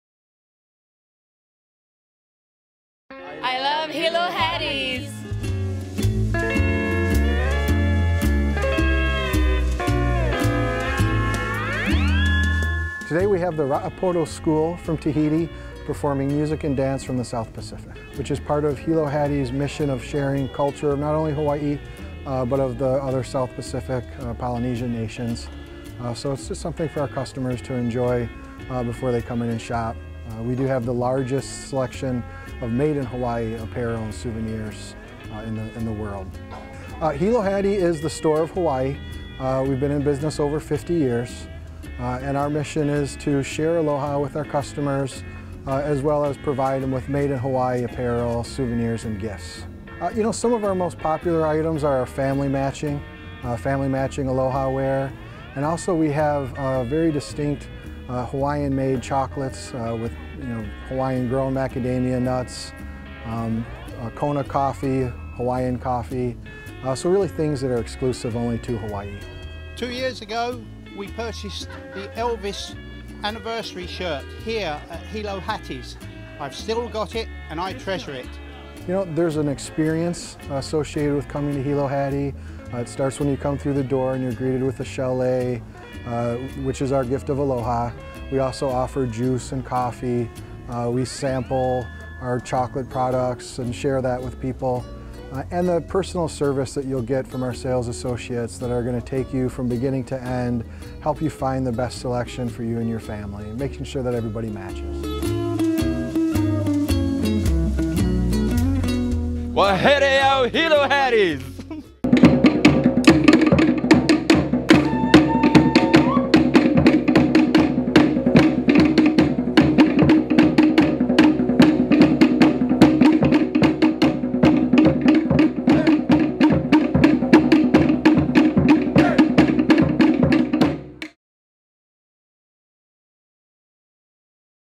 Stirling Conrad Interview Hilo Hattie Hilo Hattie Outlet Tahiti Raapoto School Performance
Interview with Stirling Conrad, director of retail for the Neighbor Islands at the Hilo Hattie Outlet at the Outlets of Maui Lahaina. Includes a performance from the Lycee Samuel Raapoto School of Tahiti. Filmed and Edited by Frank W. Pulaski III, DreamTyme Films